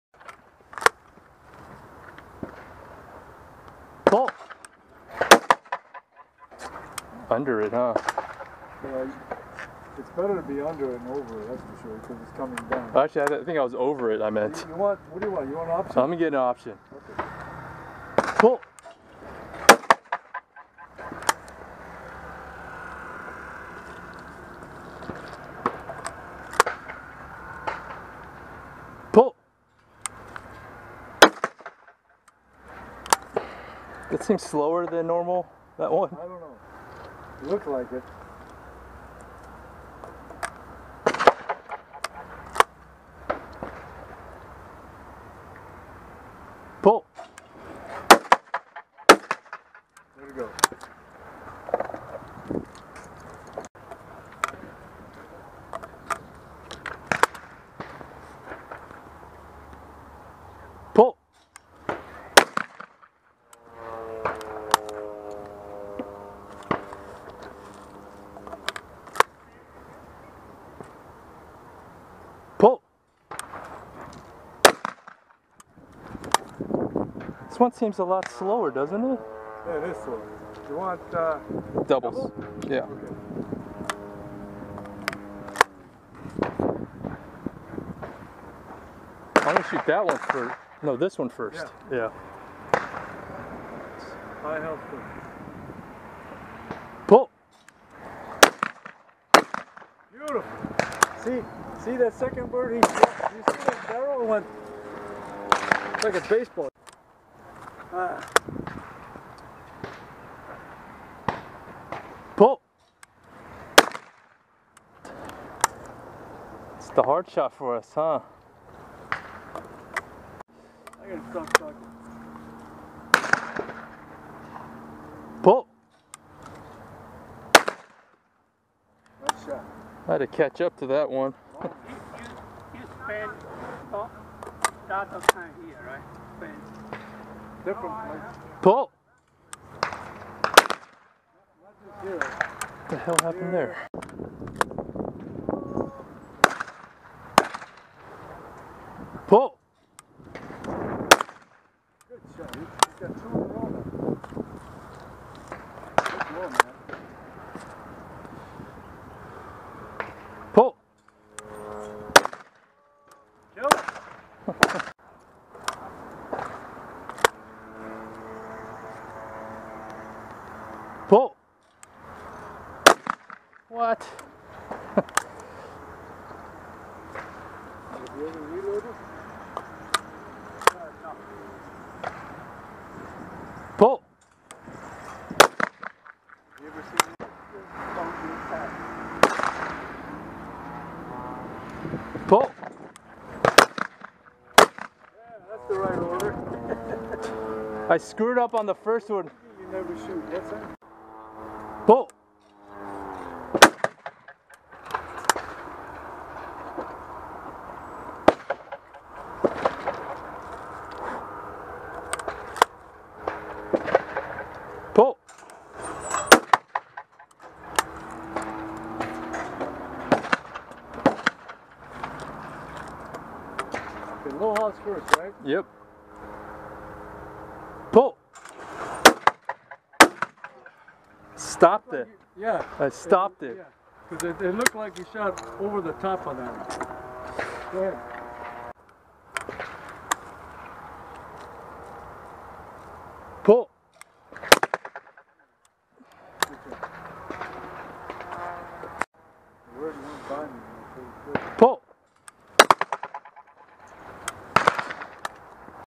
Preseason Warm Up
In preparation for the upcoming bird season we took to the skeet fields with my Merkel 147SL 12 gauge.  Need to shake the rust off.